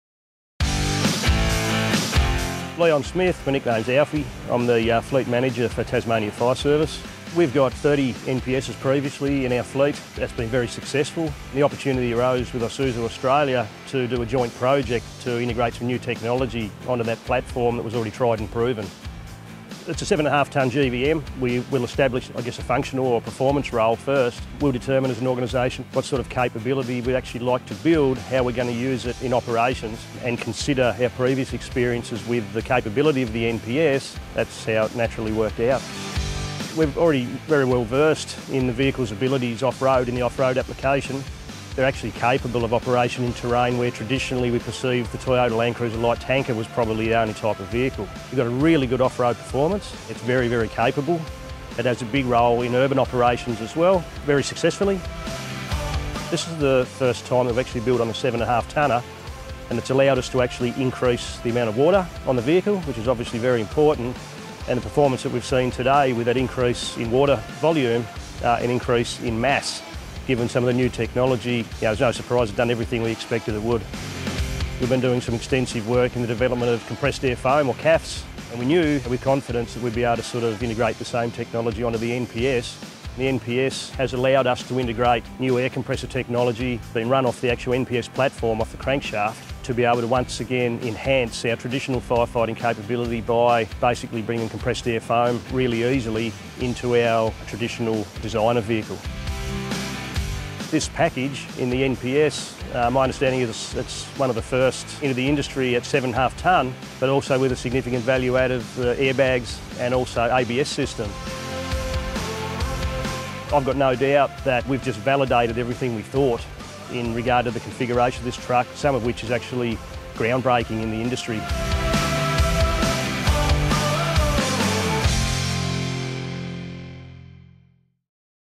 Isuzu Trucks: Tasmania Fire Service - Isuzu Australia Limited
Tasmania Fire Service's Leon Smith talks us through their brand new Isuzu NPS 300 4x4 Crew with unique compressed foam technology (CAFS).